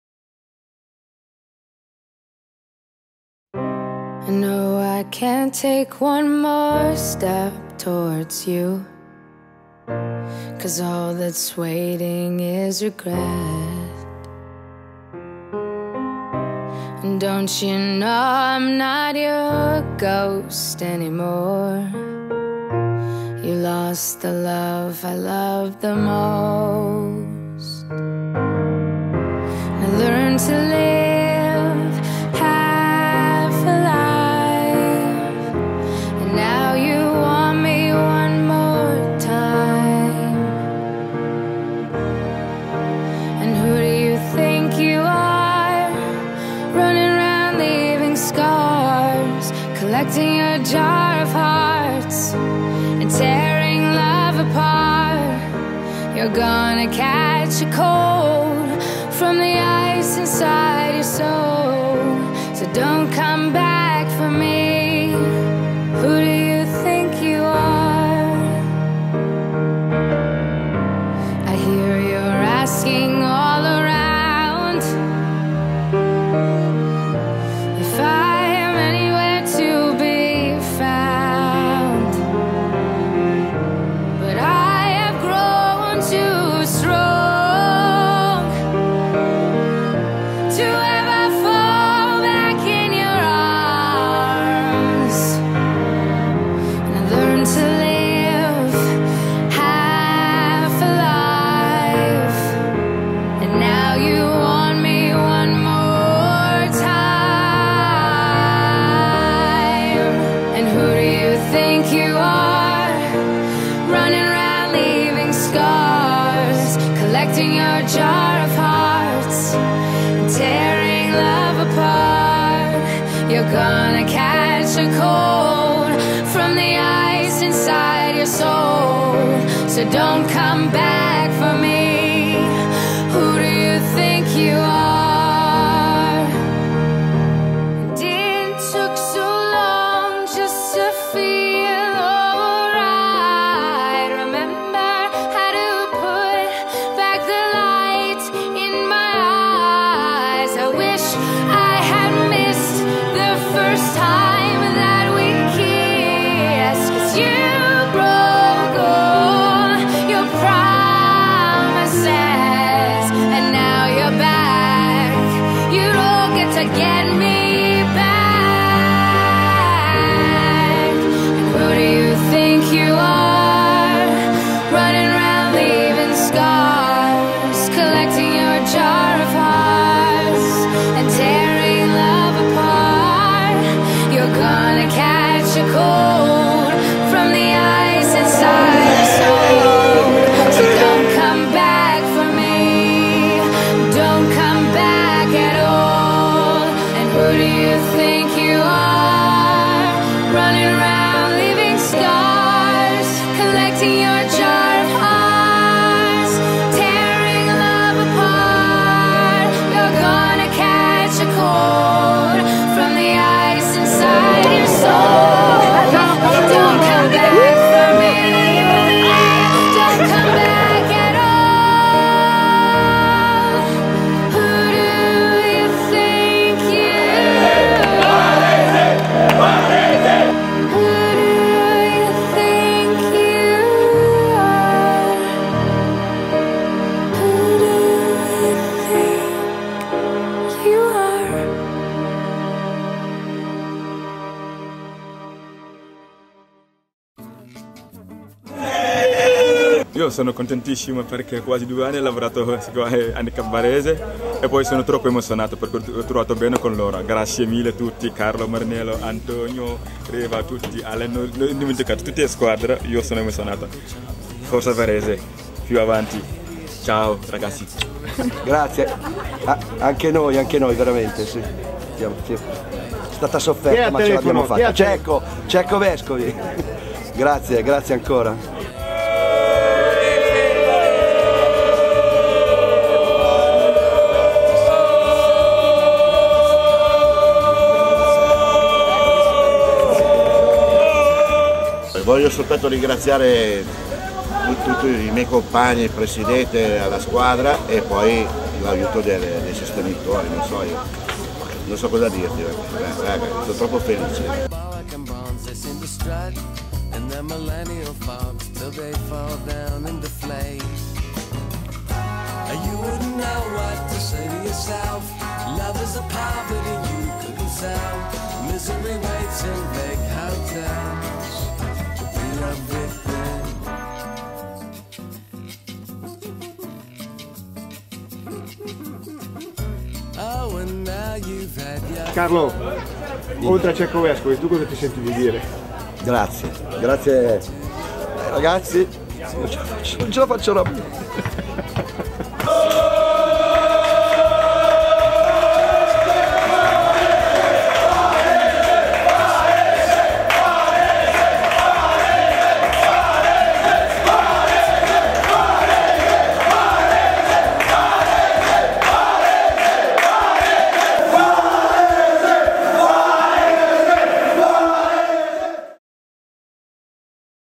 Un Bof. Handicap Sport Varese in serie A
La squadra di basket in carrozzina Handicap Sport Cimberio Varese festeggia i suoi 30 anni di storia conquistando la Serie A. Video presentato sul palco del Cinema Teatro Vela di Varese nel corso della serata "Padri di Una Buona Idea" by Sestero onlus. Varese 2014